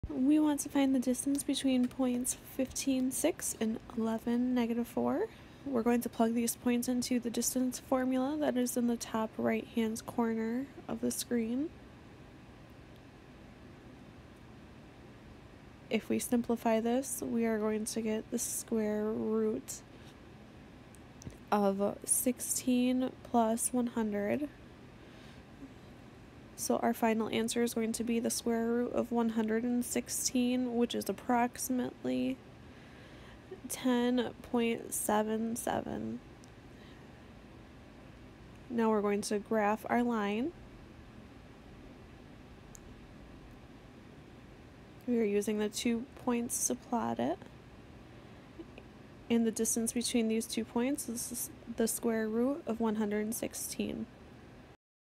Graphs: Find the distance between (15,6) and (11,-4)
Tiala from SVSU Micro Math helps you find the distance between two points using the distance formula.

Problem: Find the distance between (15,6) and (11,-4)